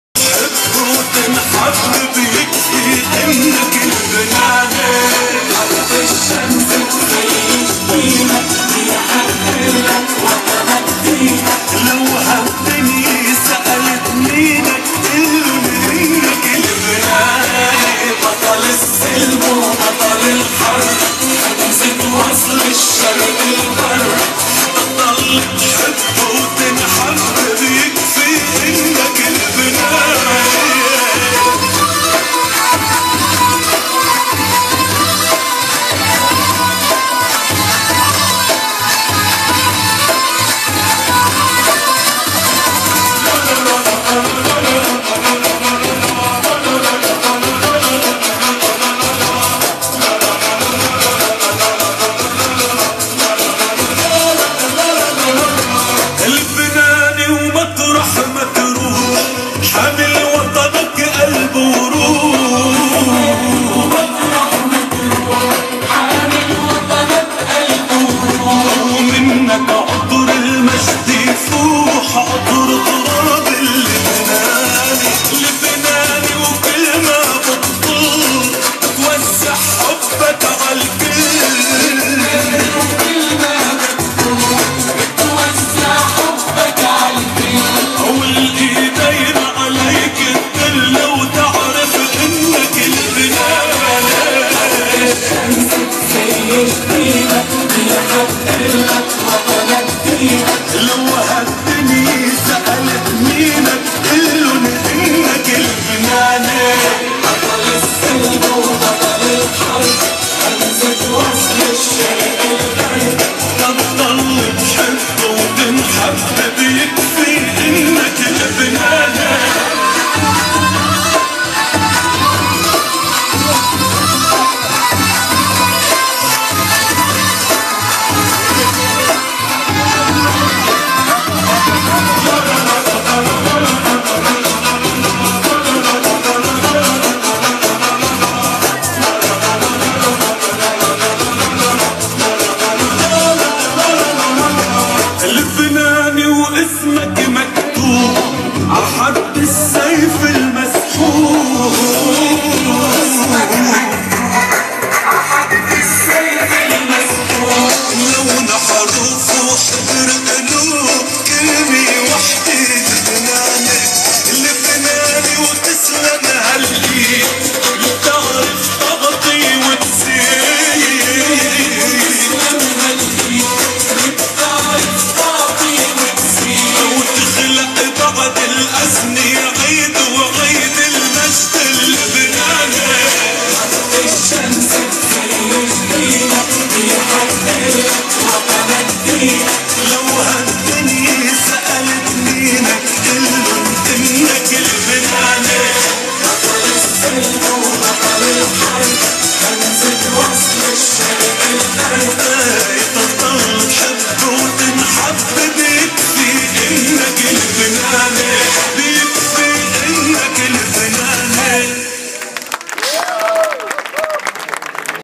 Lebanese Dabke dance
Traditional Dabke Dance at Lebanese Pavilion Global Village 2013